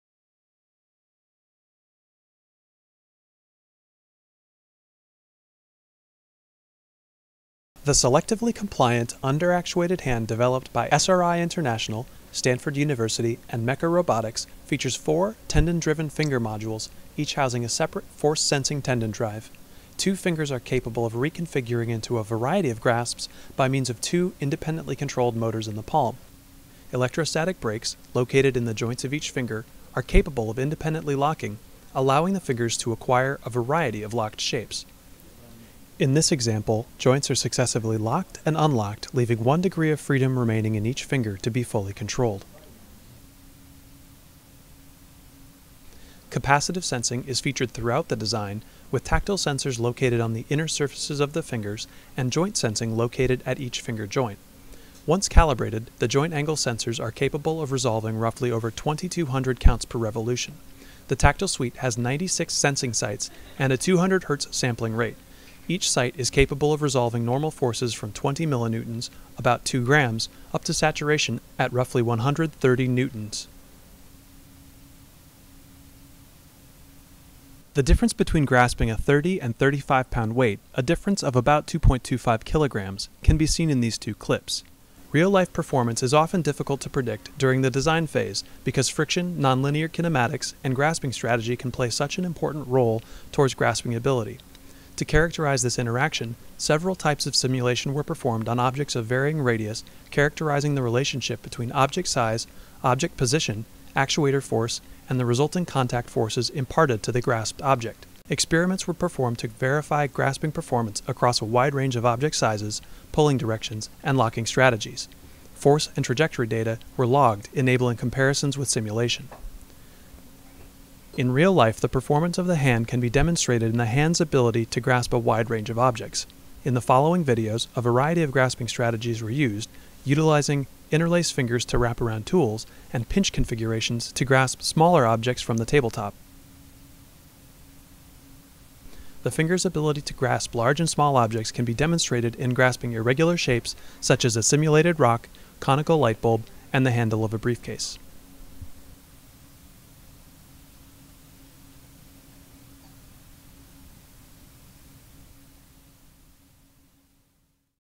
SRI/Stanford/Meka Hand for DARPA ARM-H
Overall design and tactile sensing demonstration for the under-actuated SRI/Stanford/Meka hand designed for the DARPA ARM-H program.  John Ulmen, et al., BDML Stanford